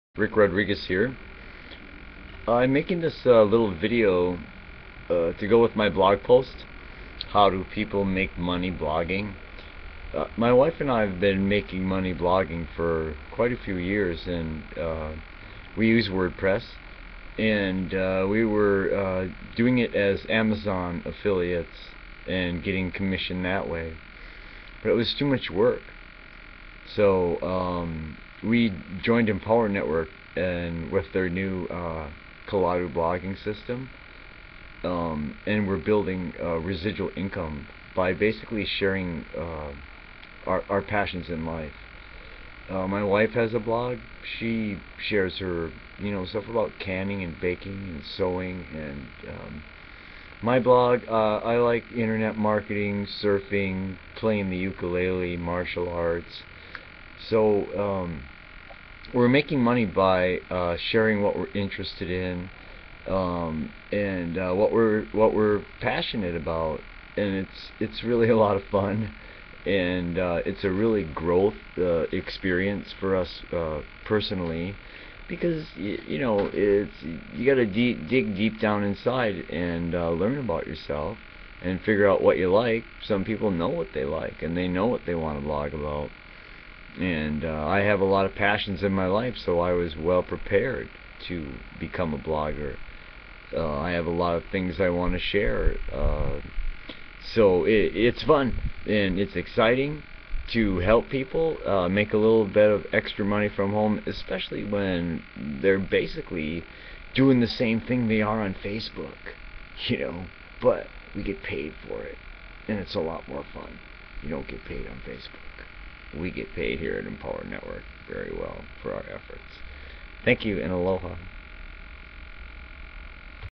How do People make Money Blogging?
How do People make Money Blogging? We make money by sharing our passions in life.  Its fun and exciting to finally be able to share with people the things that we are passionate about while earning an income.